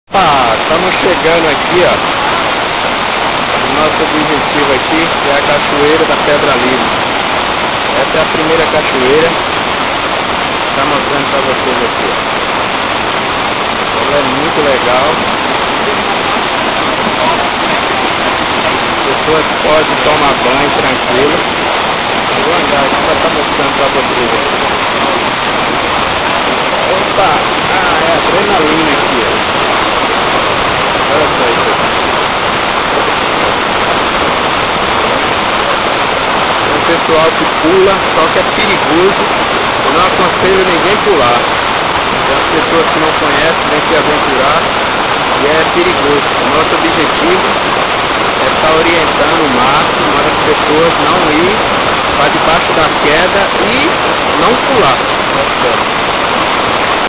cachoeira da pedra lisa
essa cachoeira é maravilhosa!! localizada no bairro de boiçucanga, é a mais linda do litoral norte!